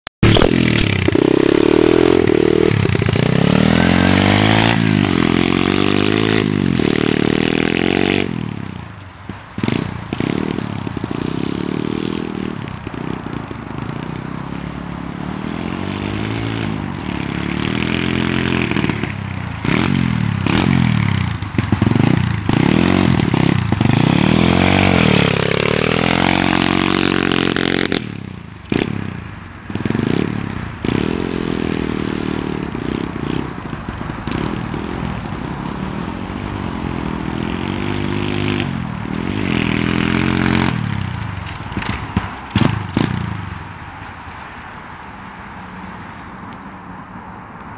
125cc stomp
video uploaded from a mobile device: my 125cc/110cc stomp runs ok first ride this is , half throttle bike builds spped gradually full throttle then wants to bog out any help please .. thanks .